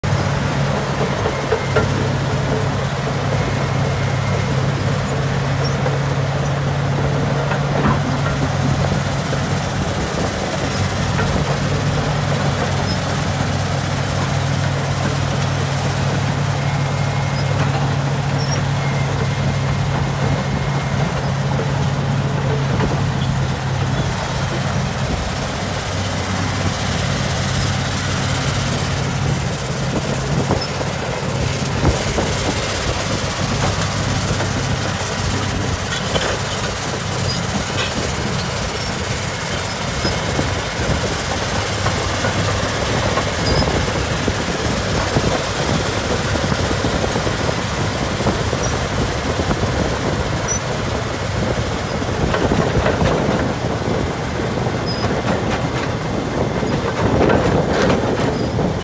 CAT D10R CRAWLER TRACTOR
CAT D10R CRAWLER TRACTOR SN:3KR01216 powered by Cat 3412E diesel engine, equipped with EROPS, air, 10U/tilt blade, 3rd valve, 4 barrel single shank ripper w/ pin puller, 28in. SBG pads.